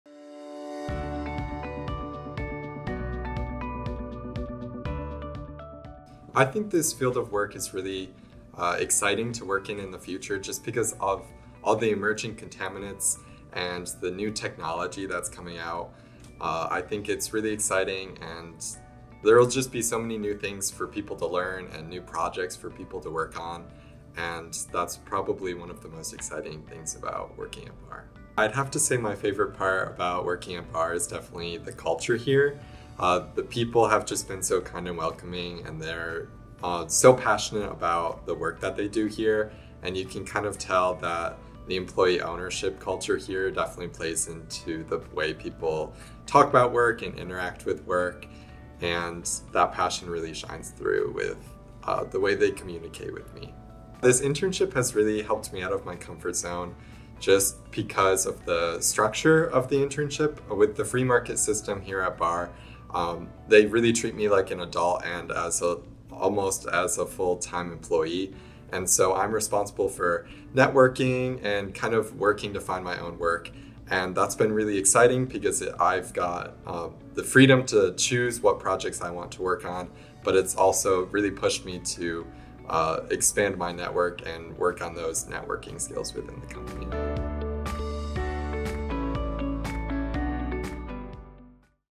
Intern Spotlight | Jaron
Jaron Frank is an environmental engineering intern currently studying at the University of Minnesota. Join us as we learn about Jaron’s experience in our Minneapolis office and what you can expect as an intern at Barr!